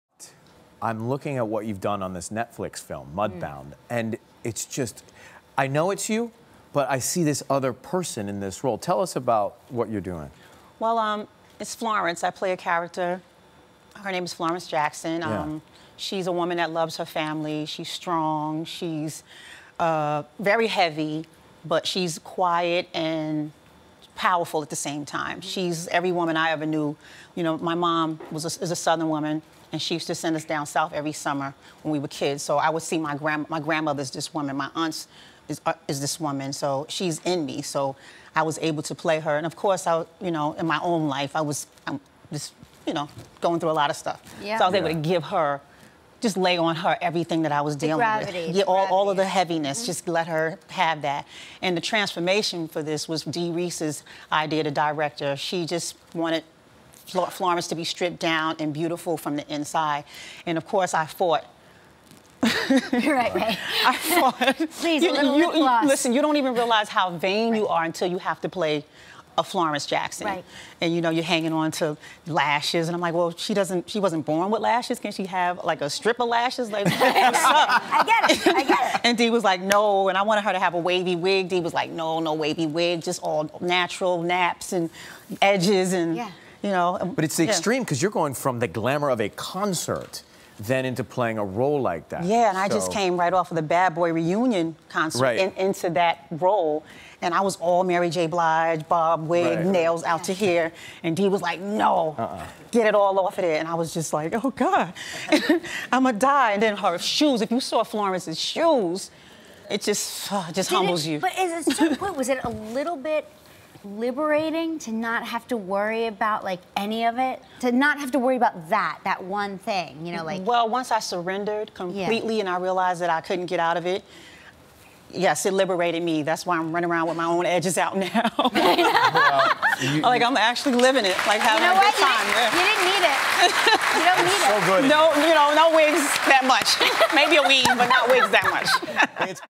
Mary J. Blige on Not Wearing Makeup In "Mudbound"
Mary J. Blige talks about her role in "Mudbound" and the choice to play her character without any makeup.